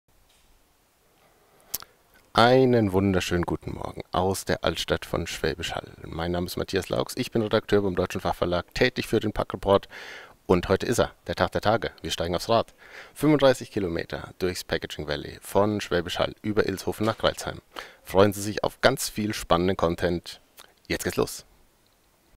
Tour de Packaging Valley
Unser Redakteur Matthias Laux freut sich auf Livebegegnungen mit der Branche und fährt mit dem Rad von Schwäbisch Hall nach Crailsheim durchs Packaging Valley.  Gesamtlänge ca. 35 km. Mit Stopps bei der OPTIMA packaging group GmbH, Packaging Valley Germany e.V., Bausch+Ströbel und der Gerhard Schubert GmbH. Erste Eindrücke.